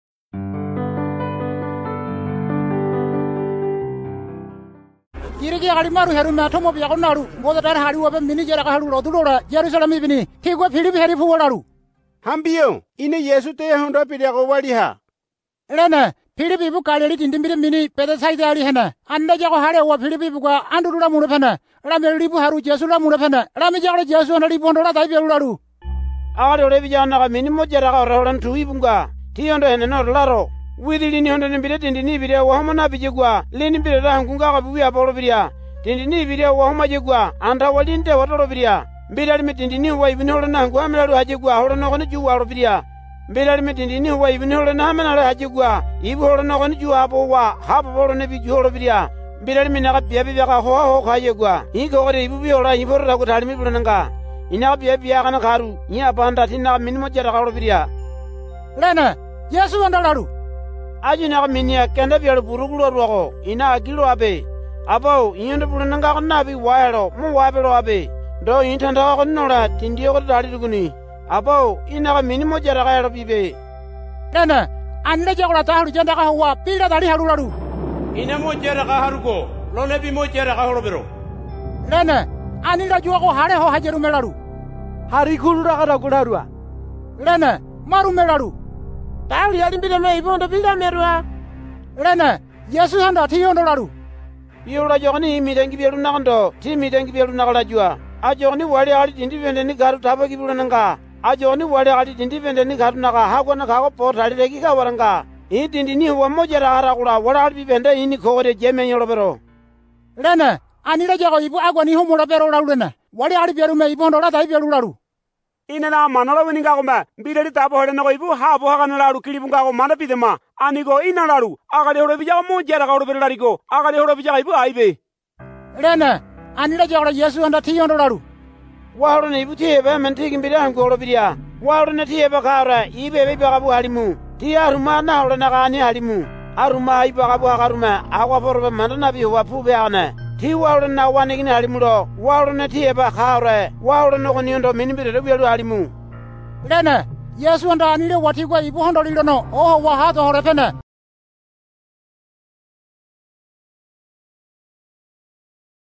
Huli - John 12:20-36 - Jesus foretells his death [hui]
Scripture Videos from the Gospel of John in Huli of Papua New Guinea. [hui]